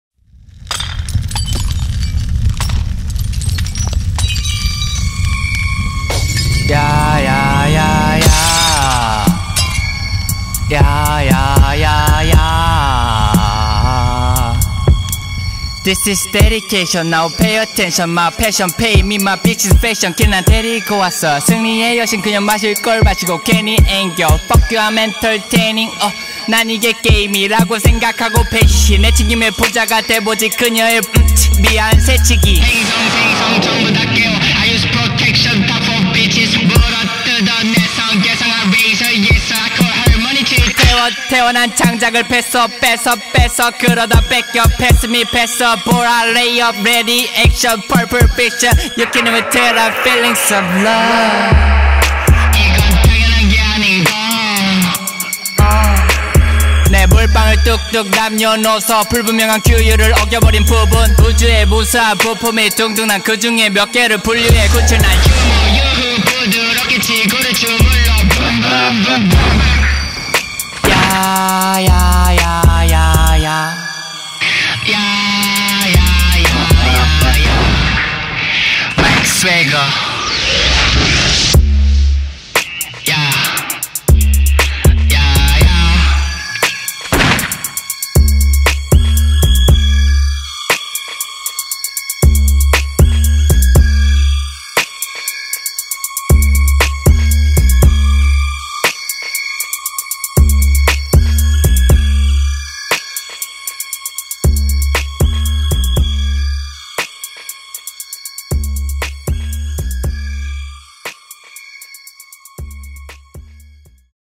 [New Era x MIC SWG] BPM EP08 Dbo 디보 (Beats By Minor Soul aka 김백수 in 어른이TV)
본격전문피규어방송 [어른이TV] 의 김백수입니다.

어른이TV 다음편은 열심히 편집중입니다.
아마 오늘이나 내일 쯤 올라가겠네요;

네 뭐...
요새 나름 재미를 붙여 열심히 참가하고 있는
마이크 스웨거의 비트입니다-.-

사실 클라우드 스타일을 그다지 좋아하지 않아서;;;
살짝 고민을 하긴 했습니다만
뭐 어쩌겠습니까 이게 유행이라는데...

ㅎ ㅏ..
이젠 힙합에서조차 세대차이를 느껴버립니다그려...

그렇잖아요
클럽을 가도 내가 좋아하는 노래는
하나도 안나와!
ㅠ_ㅠ...

아무튼 뭐..
랩만 딱 듣고선 대충 다들 어떻게들 하겠구나 감이 오길래
좀 다르게 해볼까 했으나.

뭐 그냥 했습니다-.-
재미로 들어주시면 좋겠습니다 -ㅠ-

오늘은 로또가 되야 할텐데...--);;